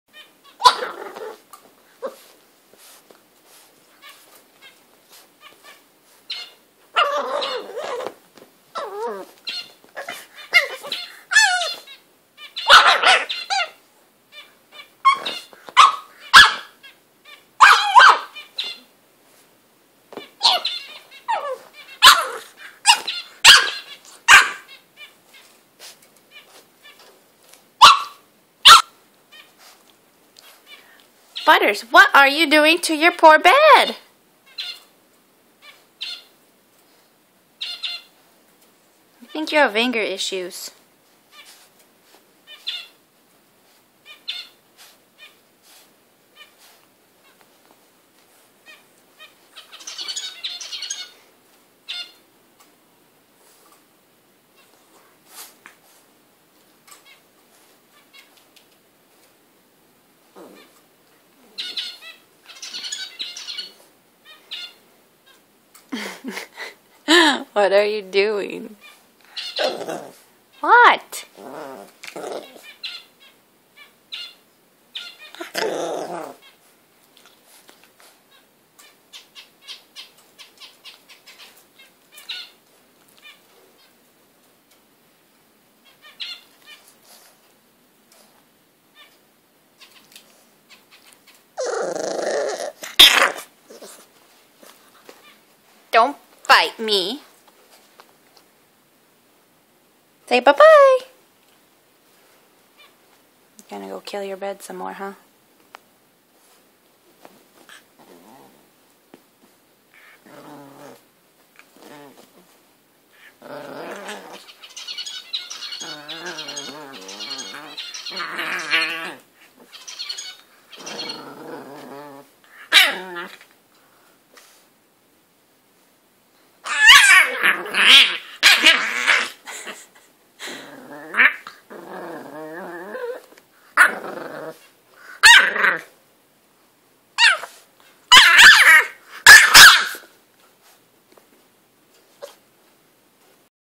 Butters is a wierdo!
My English bulldog being a bully to his bed!